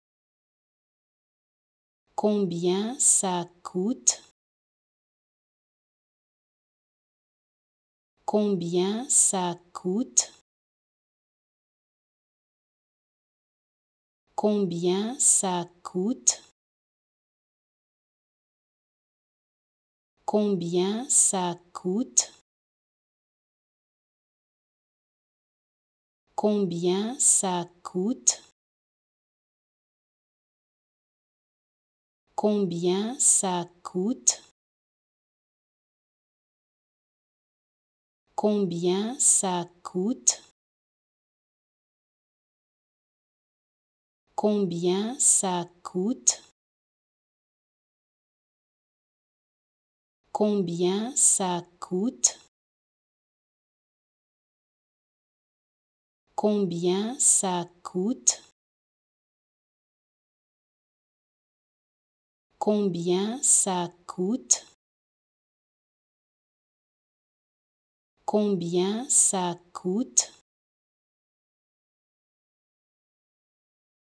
How to Pronounce ''Combien ça coûte?'' (How much does it cost?) Correctly in French
Learn how to say and properly pronounce ''Combien ça coûte?'' in French with this free pronunciation tutorial. Apprenez à prononcer des mots en français grâce à nos tutoriels.